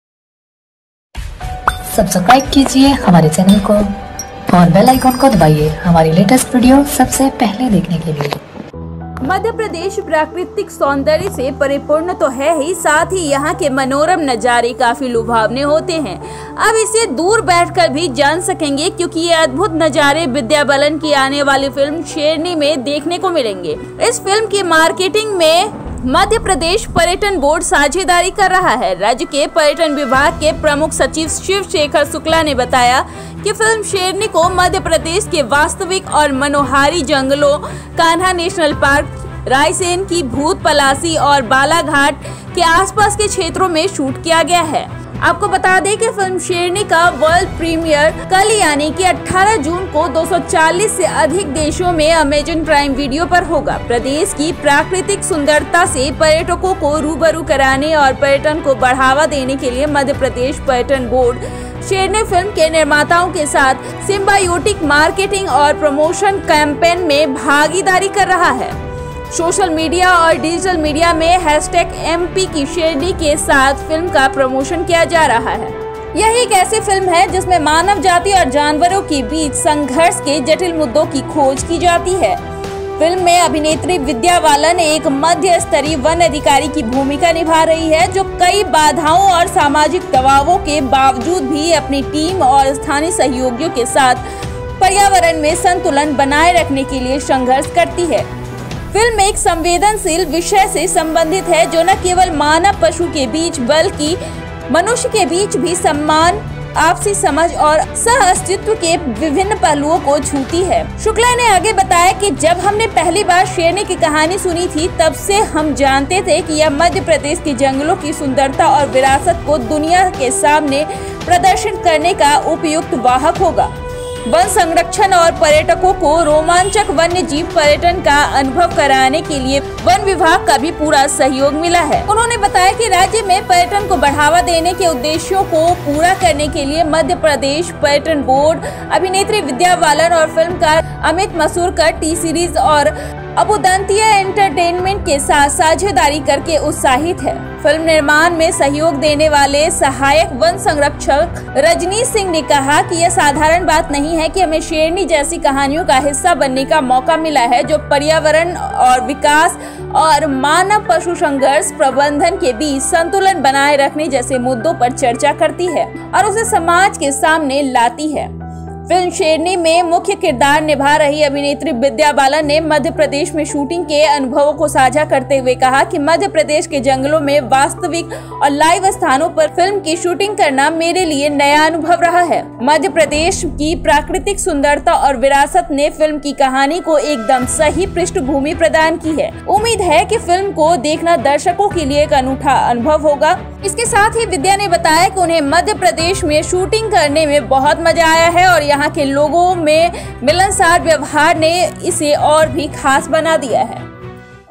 Vidya Balan's film 'Sherni' movie amazing views of MP will fascinate the mind || MP Turism
मध्य प्रदेश (Madhya Pradesh) प्राकृतिक सौंदर्य से परिपूर्ण तो है ही, साथ ही यहां के मनोरम नजारे काफी लुभावने है. अब इसे दूर बैठकर भी जान सकेंगे, क्योंकि ये अद्भुत नजारे विद्या बालन की आने वाली फिल्म ‘शेरनी’ में देखने को मिलेंगे. इस फिल्म की मार्केटिंग में मध्य प्रदेश पर्यटन बोर्ड साझेदारी कर रहा है. राज्य के पर्यटन विभाग के प्रमुख सचिव शिव शेखर शुक्ला ने बताया, “फिल्म ‘शेरनी’ को मध्यप्रदेश के वास्तविक और मनोहारी जंगलों, कान्हा नेशनल पार्क, रायसेन के भूत पलासी और बालाघाट के आसपास के क्षेत्रों में शूट किया गया है.”

आपको बता दें कि फिल्म ‘शेरनी’ का वर्ल्ड प्रीमियर कल यानी 18 जून को 240 से अधिक देशों में एमेजॉन प्राइम वीडियो पर होगा. प्रदेश की प्राकृतिक सुंदरता से पर्यटकों को रू-ब-रू कराने और पर्यटन को बढ़ावा देने के लिए मध्य प्रदेश पर्यटन बोर्ड, ‘शेरनी’ फिल्म के निमार्ताओं के साथ सिंबायोटिक मार्केटिंग और प्रमोशन कैंपेन में भागीदारी कर रहा है. सोशल मीडिया और डिजिटल मीडिया में MPKiSherni के साथ फिल्म का प्रमोशन किया जा रहा है.
फिल्म के साथ-साथ एमपी के पर्यटन स्थलों का प्रमोशन
यह ऐसी फिल्म है, जिसमें मानव जाति और जानवरों के बीच संघर्ष के जटिल मुद्दों की खोज की जाती है. फिल्म में अभिनेत्री विद्या बालन एक मध्य स्तरीय वन अधिकारी की भूमिका निभा रही हैं, जो कई बाधाओं और सामाजिक दबावों के बावजूद भी अपनी टीम और स्थानीय सहयोगियों के साथ पर्यावरण में संतुलन बनाए रखने के लिए संघर्ष करती है. फिल्म एक संवेदनशील विषय से संबंधित है, जो न केवल मानव-पशु के बीच बल्कि मनुष्य के बीच भी सम्मान, आपसी समझ और सहअस्तित्व के विभिन्न पहलुओं को छूती हैं.

शुक्ला ने आगे बताया, “जब हमने पहली बार ‘शेरनी’ की कहानी सुनी थी, तब से हम जानते थे कि यह मध्य प्रदेश के जंगलों की सुंदरता और विरासत को दुनिया के सामने प्रदर्शित करने का उपयुक्त वाहक होगा. वन संरक्षण और पर्यटकों को रोमांचक वन्यजीव पर्यटन का अनुभव कराने के लिए वन विभाग का भी पूरा सहयोग मिला है.”

इंसानों और पशुओं के बीच संतुलन रखने का मुद्दा उठाती है फिल्म
उन्होंने बताया कि राज्य में पर्यटन को बढ़ावा देने के उद्देश्यों को पूरा करने के लिए मध्य प्रदेश पर्यटन बोर्ड अभिनेत्री विद्या बालन और फिल्मकार अमित मसुरकर, टी-सीरीज और अबुदंतिया एंटरटेनमेंट के साथ साझेदारी करके उत्साहित है. फिल्म निर्माण में सहयोग देने वाले सहायक वन संरक्षक रजनीश सिंह ने कहा कि यह साधारण बात नहीं हैं कि हमें ‘शेरनी’ जैसी कहानियों का हिस्सा बनने का मौका मिला है, जो पर्यावरण एवं विकास और मानव-पशु संघर्ष प्रबंधन के बीच संतुलन बनाए रखने जैसे मुद्दों पर चर्चा करती है और उसे समाज के सामने लाती हैं.

फिल्म ‘शेरनी’ में मुख्य किरदार निभा रही अभिनेत्री विद्या बालन ने मध्य प्रदेश में शूटिंग के अनुभवों को साझा करते हुए कहा कि मध्यप्रदेश के जंगलों में वास्तविक और लाइव स्थानों पर फिल्म की शूटिंग करना मेरे लिए नया अनुभव रहा है. मध्यप्रदेश की प्राकृतिक सुंदरता और विरासत ने फिल्म की कहानी को एकदम सही पृष्ठभूमि प्रदान की है. उम्मीद है कि फिल्म को देखना दर्शकों के लिए एक अनूठा अनुभव होगा. उन्होंने बताया कि उन्हें मध्य प्रदेश में शूटिंग करने में बहुत मजा आया और यहां के लोगों के मिलनसार व्यवहार ने इसे और भी खास बना दिया.